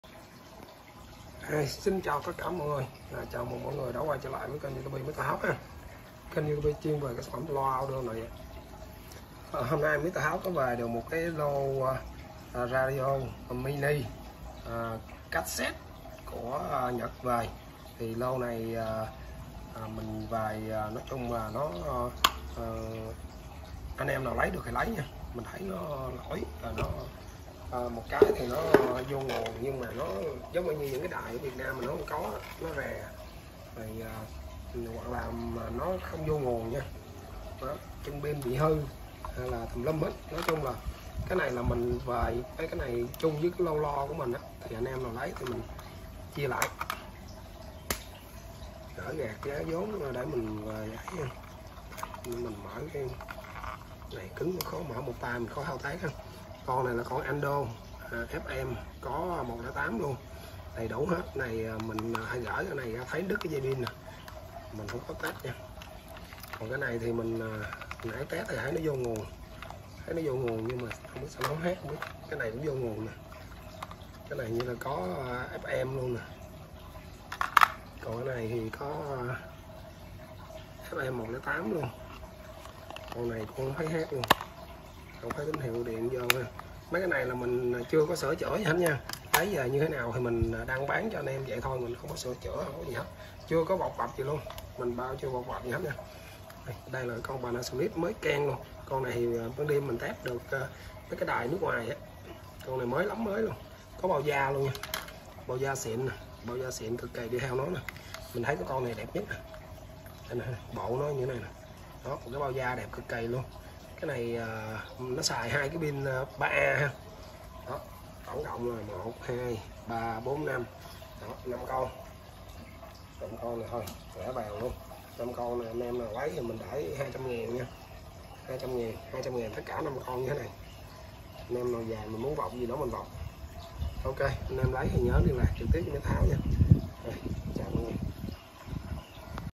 Lô radio ve chia nội địa Nhật giá rẻ
5 con radio cầm tay hàng nội địa Nhật về
Tình trạng nguyên zin chưa sửa chữa
3 con trên có nguồn
2 con dưới không thấy nguồn , chân pin gỉ sét
Giá : 200k/5 con anh em về vọc nhé
Call : Mr Tháo ( zalo O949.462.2O6 )